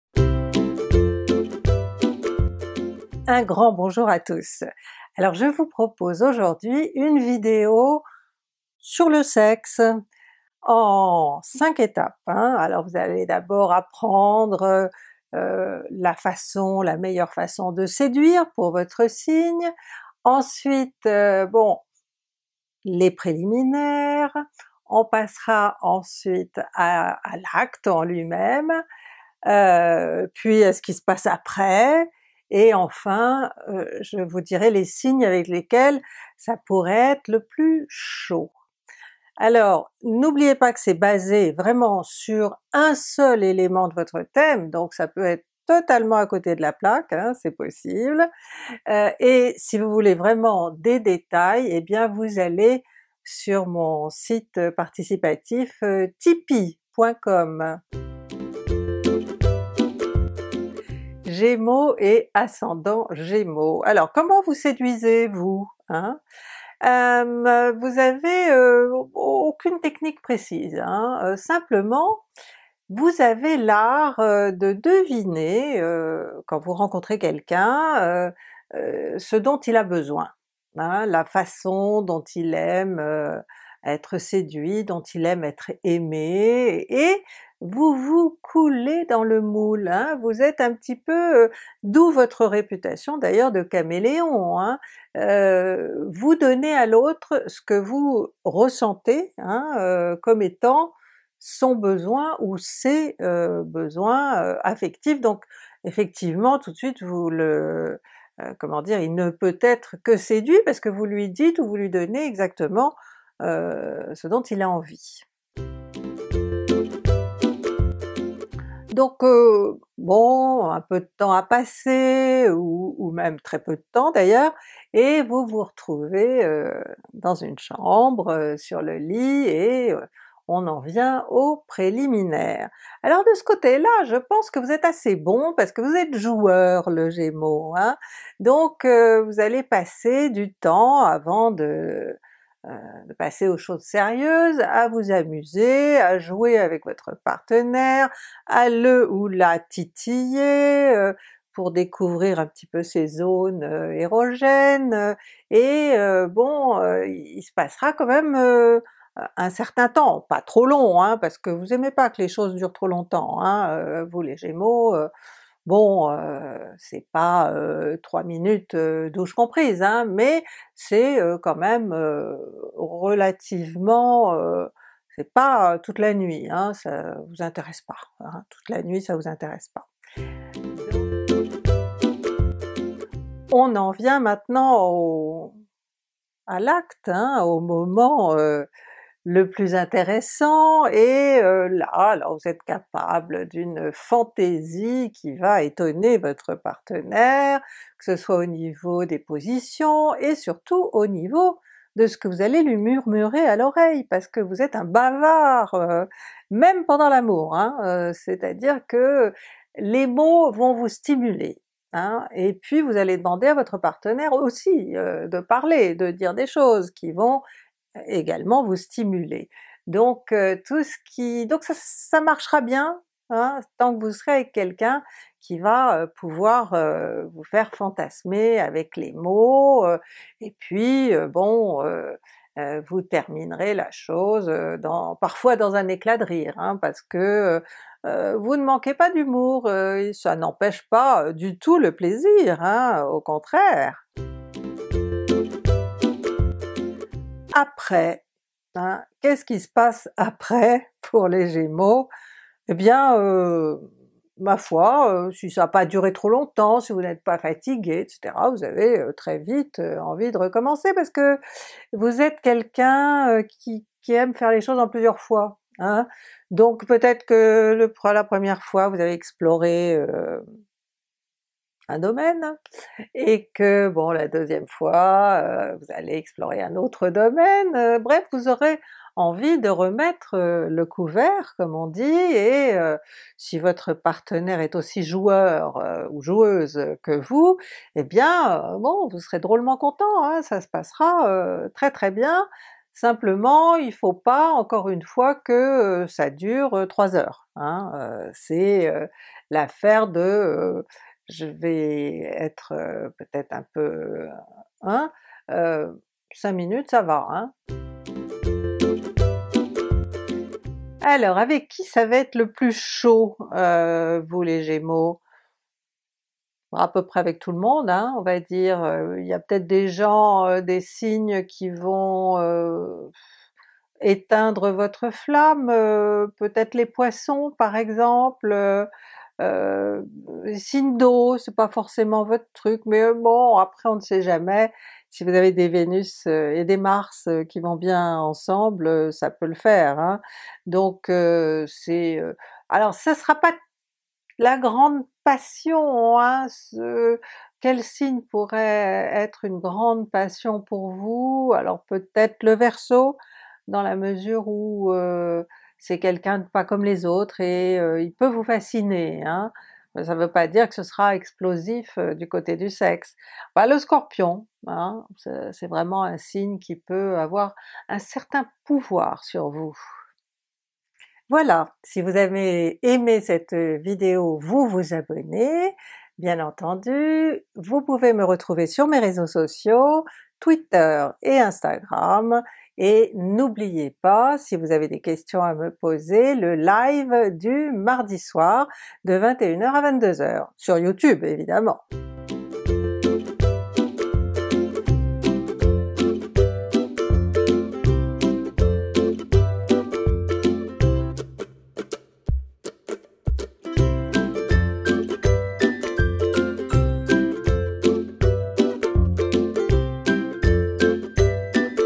Le Gémeaux et le sexe en 5 étapes 🍑🧡
🔥 Comment est le Gémeaux♊️au lit ? Je vous propose un mode d'emploi détaillé en 5 étapes afin d'en savoir plus !
ATTENTION ! Cette vidéo a été tournée il y a longtemps, ainsi le site Tipeee.com n'est plus d'actualité.